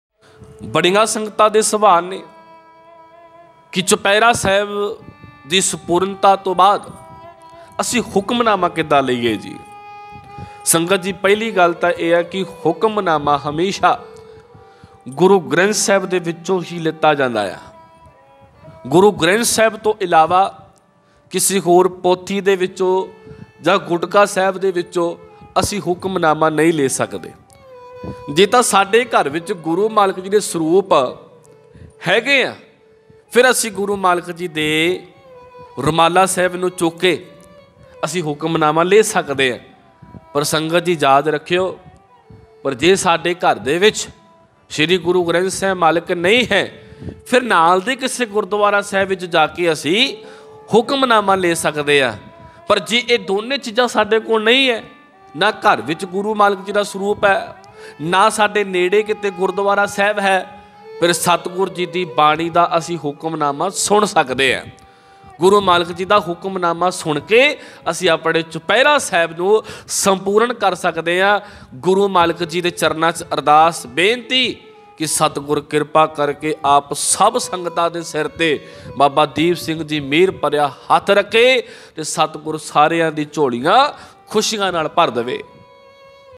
ਚੋਪਹਿਰਾ ਸਾਹਿਬ ਤੋਂ ਬਾਅਦ ਹੁਕਮਨਾਮਾ ਕਿਵੇਂ ਲਈਏ ? Chopehra sahib to baad hukamnama kiven layie ?
Title-  ਚੋਪਹਿਰਾ ਸਾਹਿਬ ਤੋਂ ਬਾਅਦ ਹੁਕਮਨਾਮਾ ਕਿਵੇਂ ਲਈਏ ? Chopehra sahib to baad hukamnama kiven layie ?
By- Karanvir Singh Bassi (Sri Fatehgarh Sahib Wale)
Label- Karanvir Singh Bassi